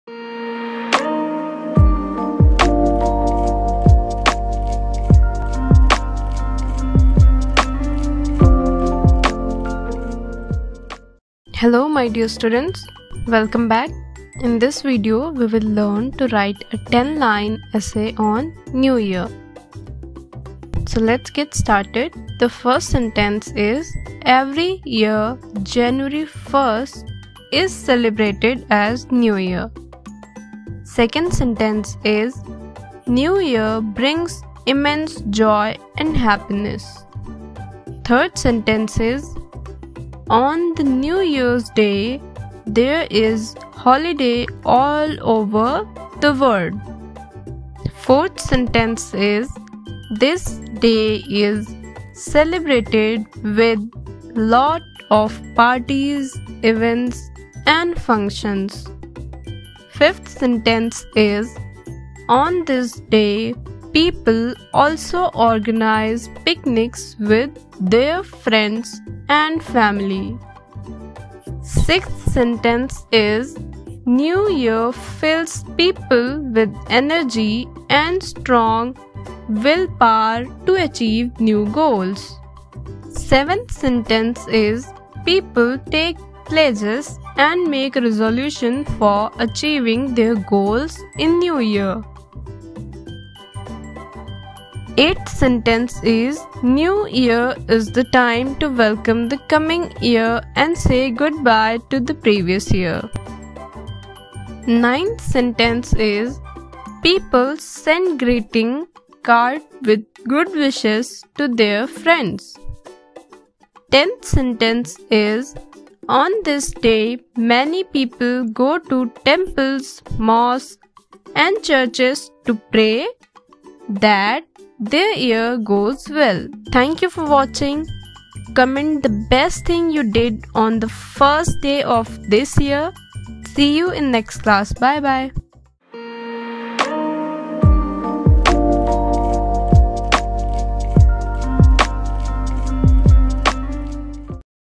10 Lines on New Year in English | Short Essay on New Year in English Writing
In this Video, I will help you to write and learn  Best 10 Lines On New Year or Short Essay on New Year in English -Learn